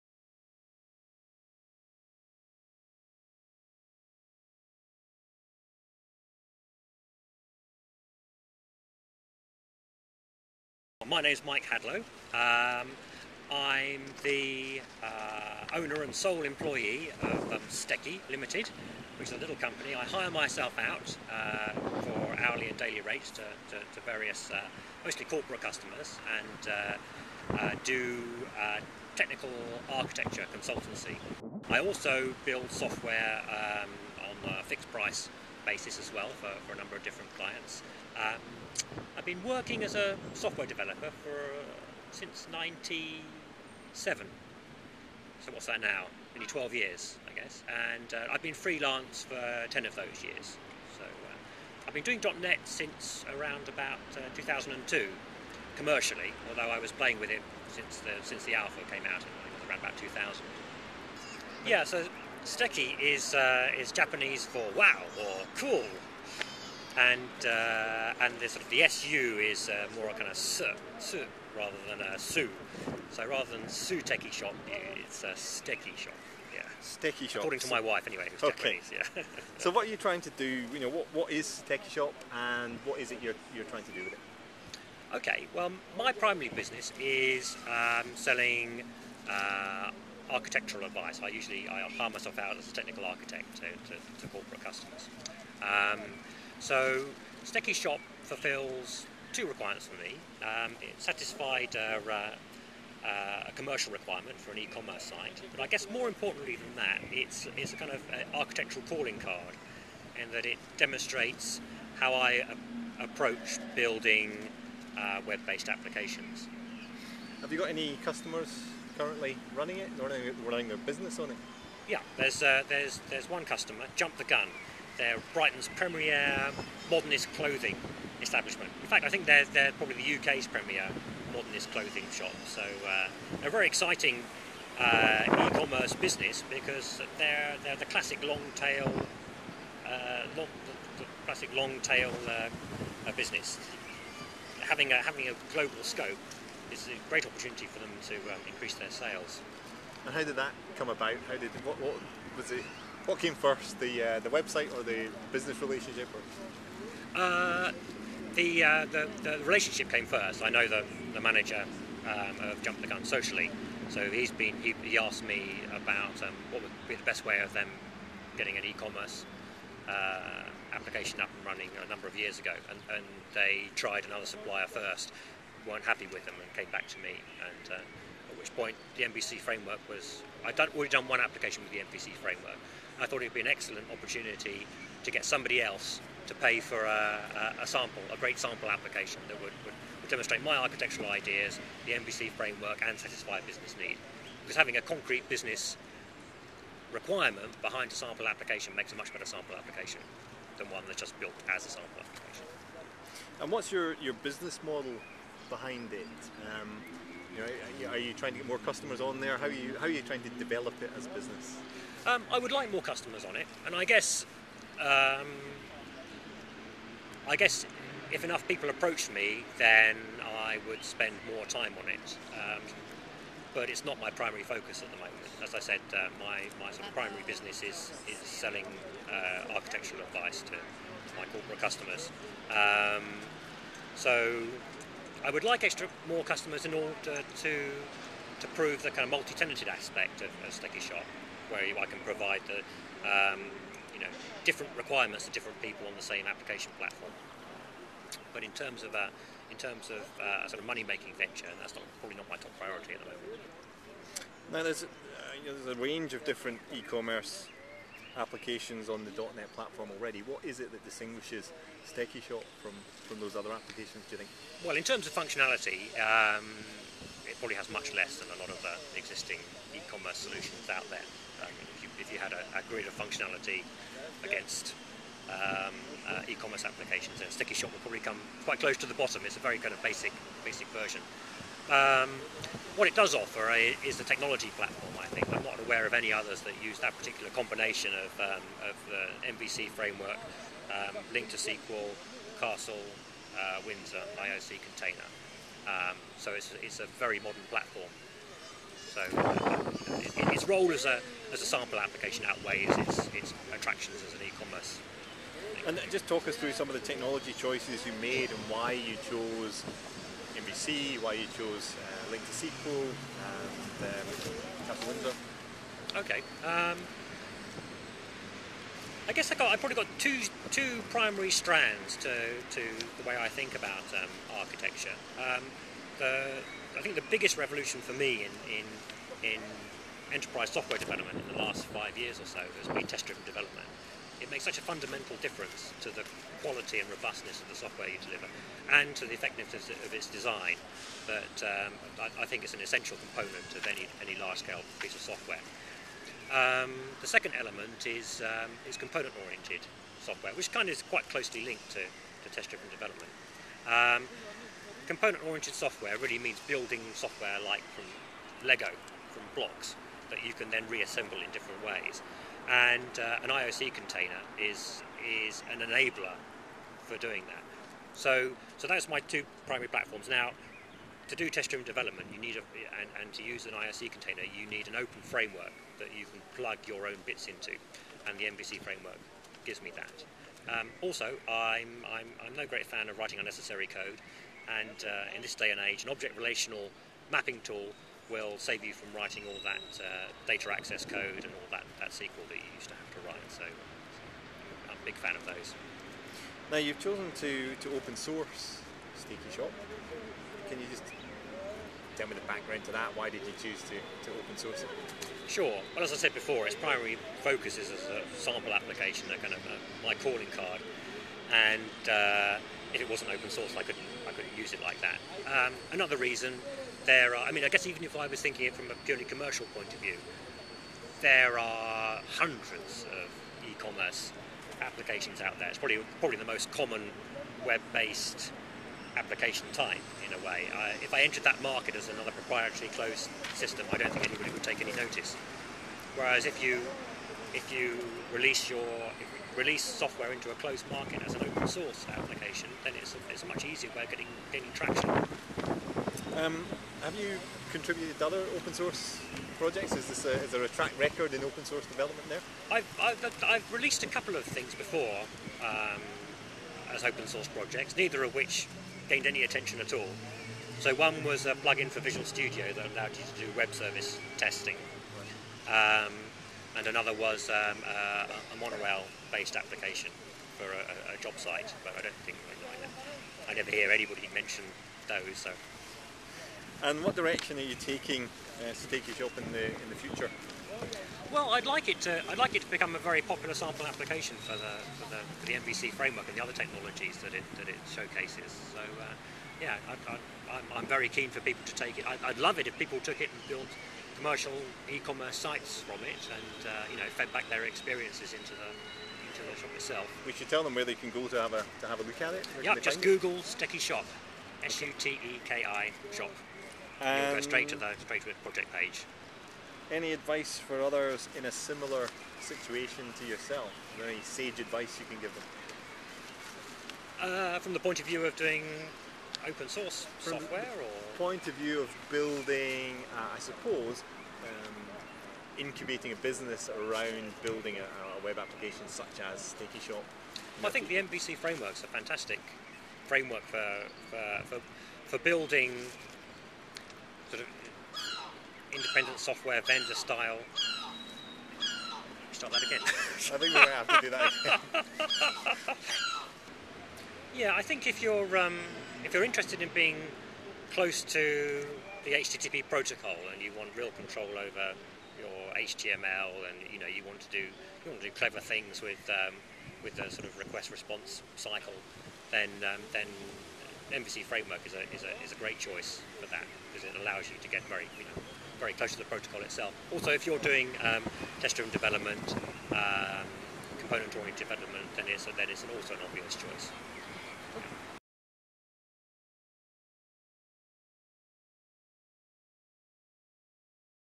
Out and About: More Mike Hadlow on ASP.NET MVC
I interview Mike Hadlow, developer of Suteki Shop and owner of Suteki Ltd. Suteki Shop is an ASP.NET MVC open source eCommerce application. We chat about "suteki", ASP.NET MVC, open source, why Suteki Shop and open frameworks.